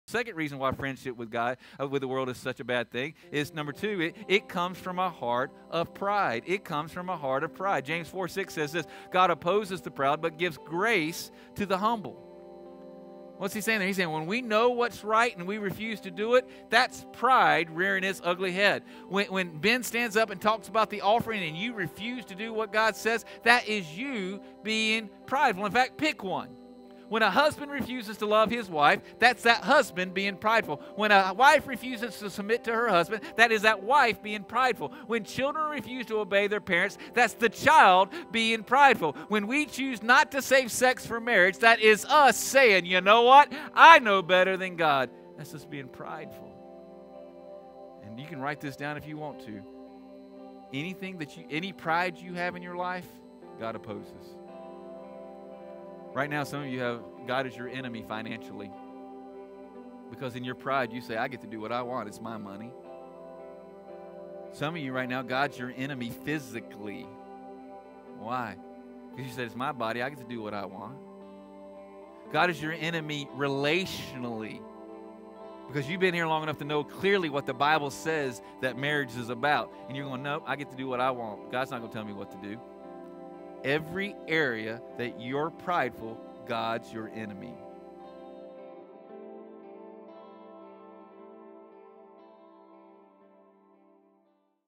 CLIP - "God OPPOSES the Proud"
In this clip from our "Enemy of God?" sermon, Pastor Randy reveals how PRIDE causes God to fight against us.